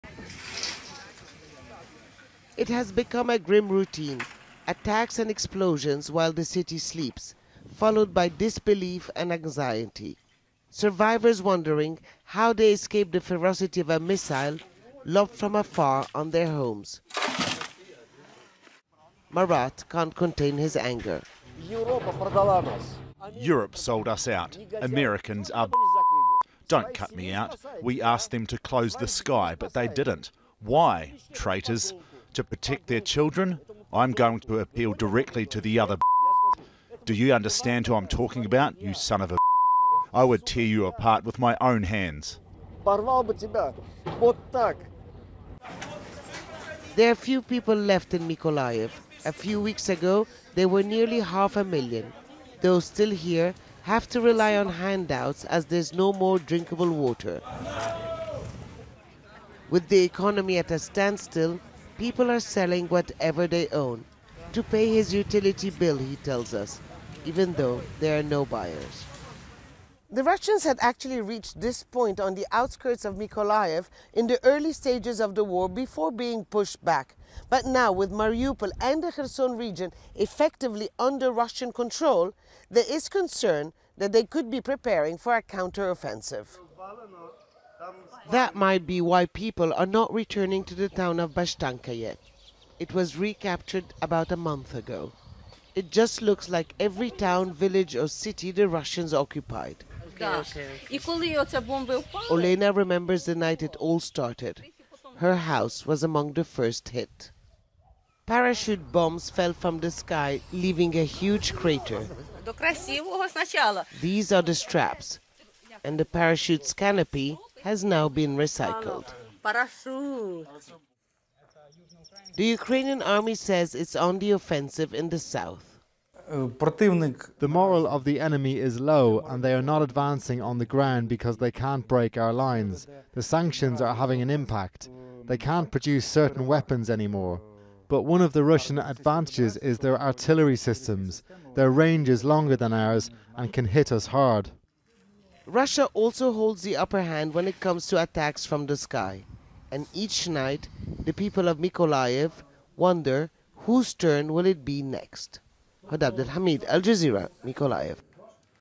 Ukraine war: Mykolaiv on edge amid fear of a new Russian assault
Russia's progress in Mariupol has sparked concerns it could widen its offensive to target more of southern Ukraine.
Ukrainian army says it is fighting back against Russian troops suffering low moral.
But Inside Mykolaiv, residents live in fear of renewed Russian assault.

Al Jazeera’s Hoda Abdel-Hamid reports from the Ukrainian city of Mykolaiv.